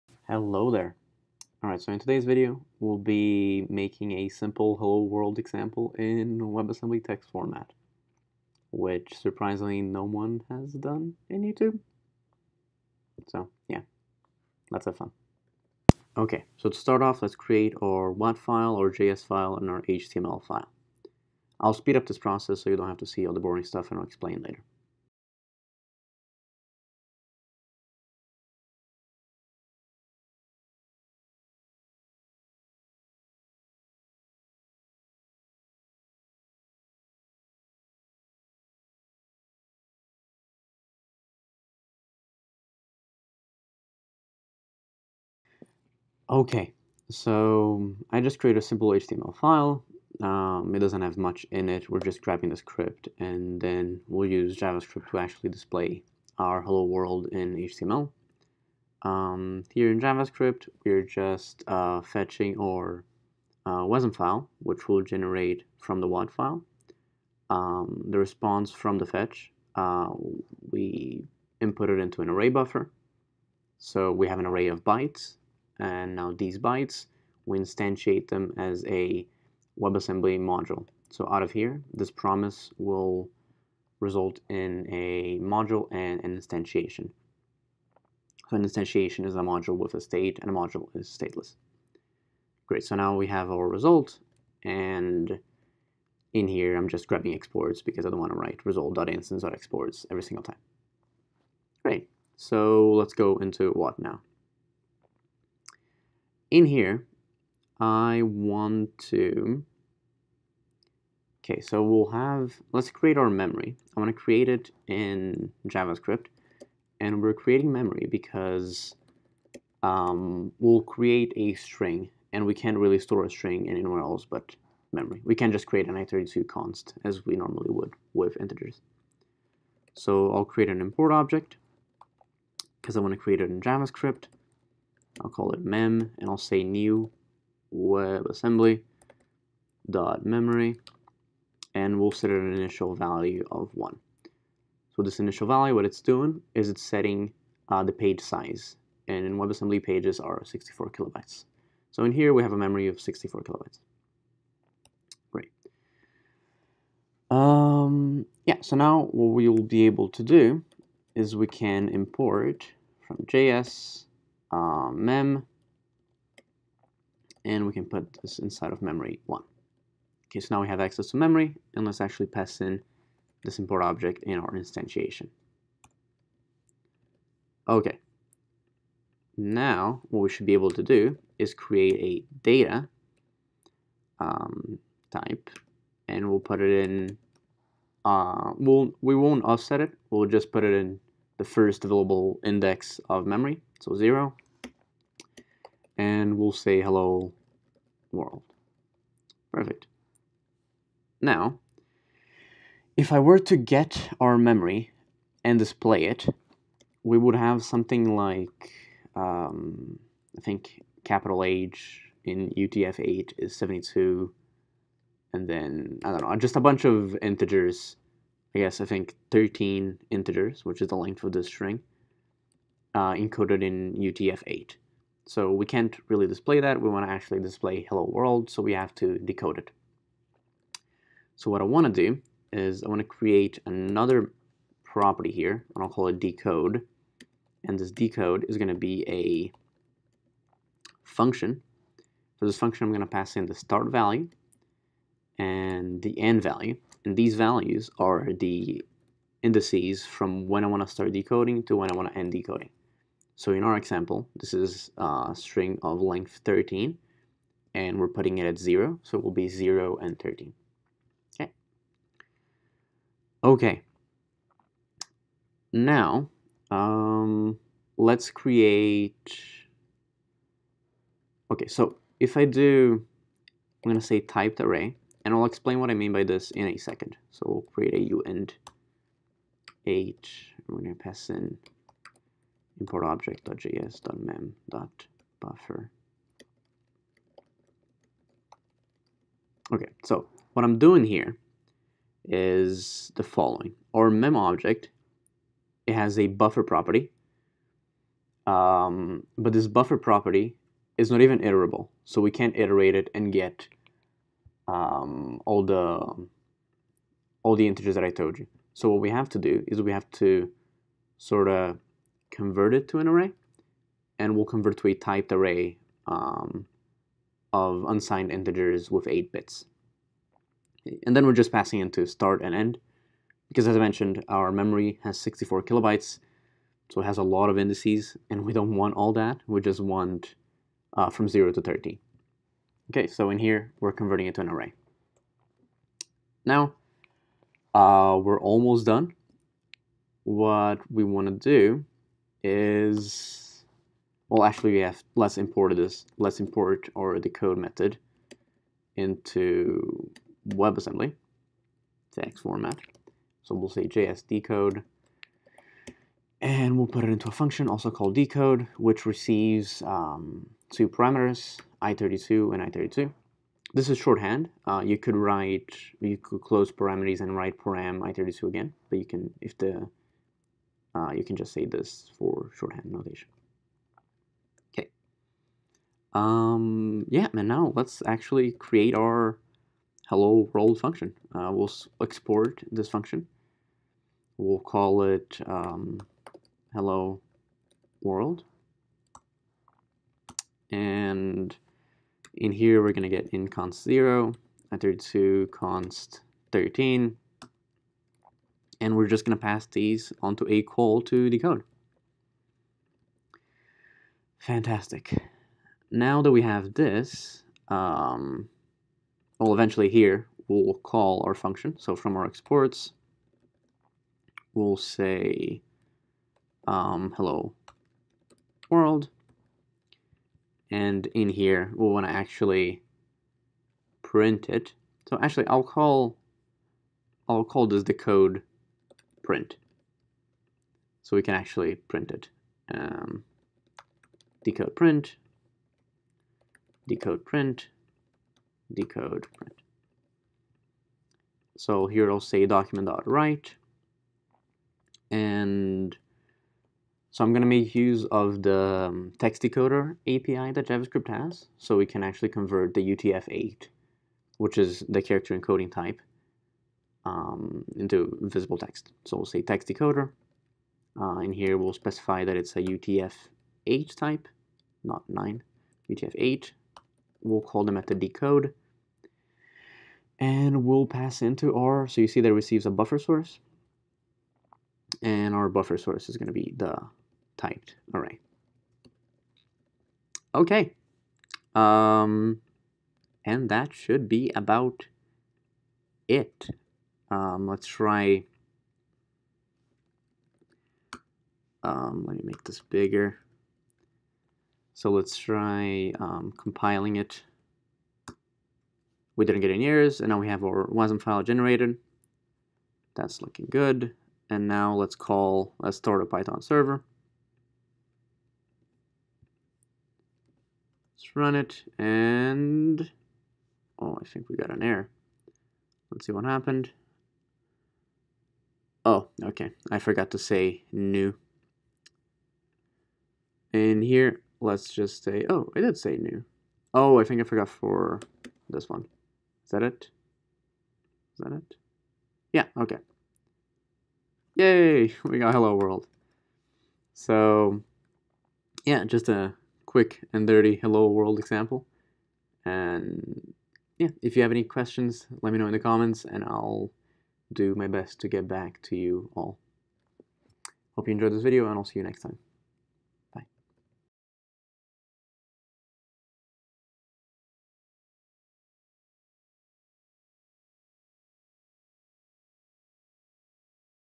"Hello, World!" in WebAssembly Text Format
WebAssembly Text Format

- Quick note:
+ After posting the video, I realized there's one other person who has made a "Hello, World!" video with WebAssembly Text Format and they do it in a very similar way to how I did it in this video (probably following Mozilla MDN too 😎). Anyway, check 'em out too

- Sources:
+ WebAssembly Text Format: